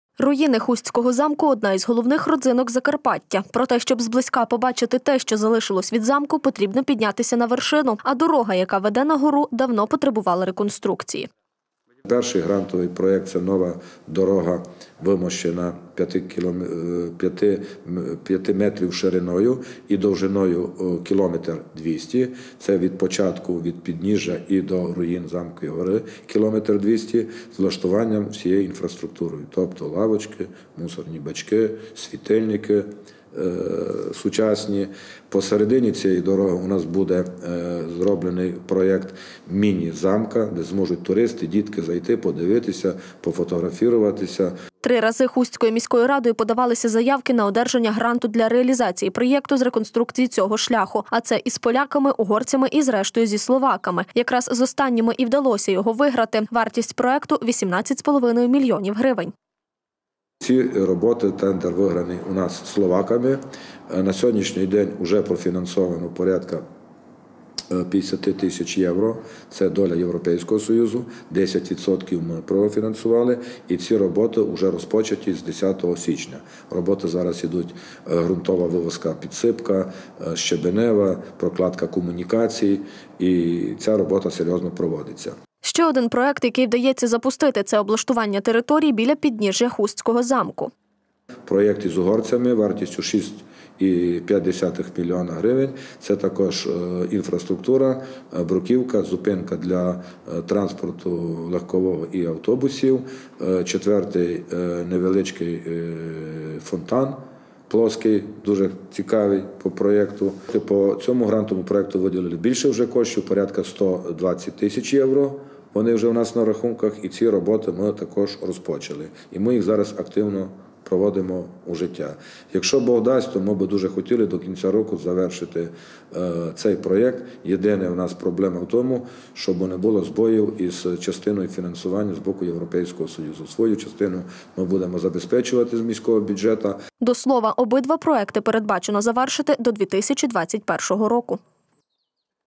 Грантові проекти
Руїни Хустського замку одна із головних родзинок Закарпаття. Проте щоб зблизька побачити те, що залишилось від замку потрібно піднятись на вершину. А дорога, яка веде на гору, давно потребувала реконструкції.Три рази Хустською міською радою подавалися заявки на одержання гранту для реалізації проєкту з реконструкції цього шляху, а це – з поляками, угорцями і зрештою, зі словаками. Якраз з останніми й вдалося його виграти. Вартість проекту – 18,5 млн гривень.Ще один проект, який вдається запустити, це облаштування території біля підніжжя Хустського замку.До слова, обидва проекти передбачено завершити до 2021 року.